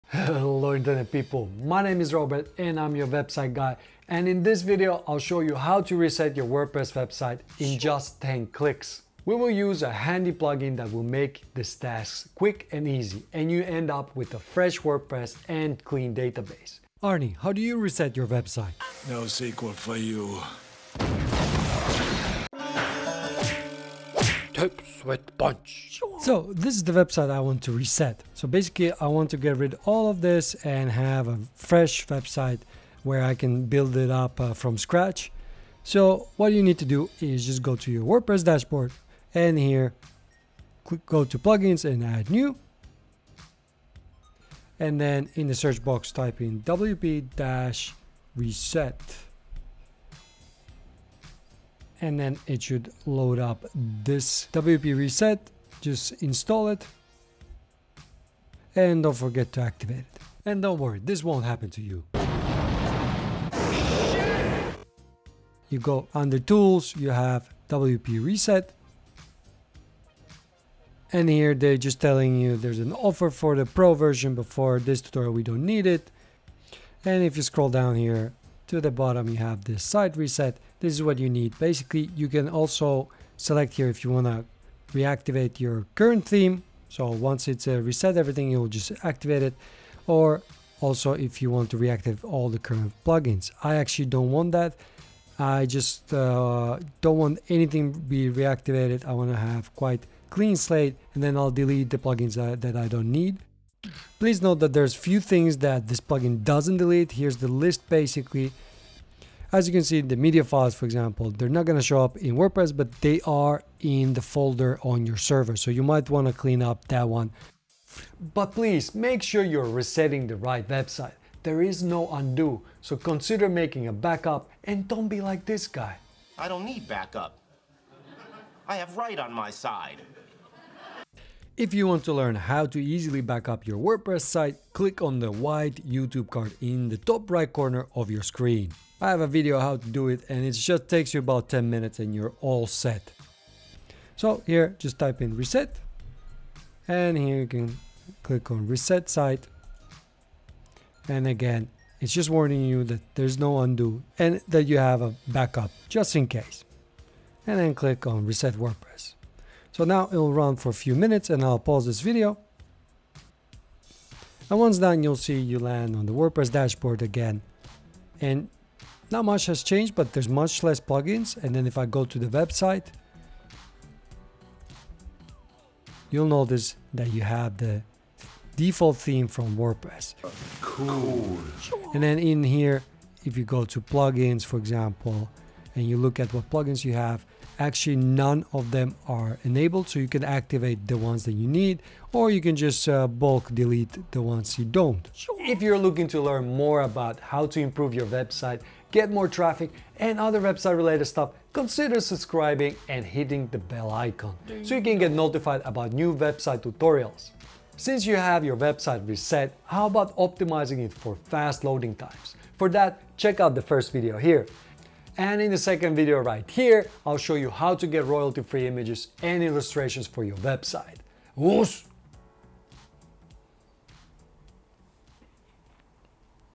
Reset WordPress Website to Default [in only 10 clicks]
I’ll show you how to reset the WordPress website back to default in just 10 clicks.
We will use a handy plugin that will make this task quick & easy and you end up with a fresh WordPress and clean database.

You can check what exactly this plugin does and doesn’t do.

But please make sure you are resetting the right website,  there is no undo, so consider making a backup.

In this tutorial, I’ll show you how to completely reset WordPress site back to its original settings. So basically you will have a default WordPress installation with blank canvas. And you can build your website from a clean sheet. So we will delete WP site and start over fresh!

Please note this plugin doesn’t delete everything, such as media files, plugins (they will be deactivated), WordPress core settings, etc.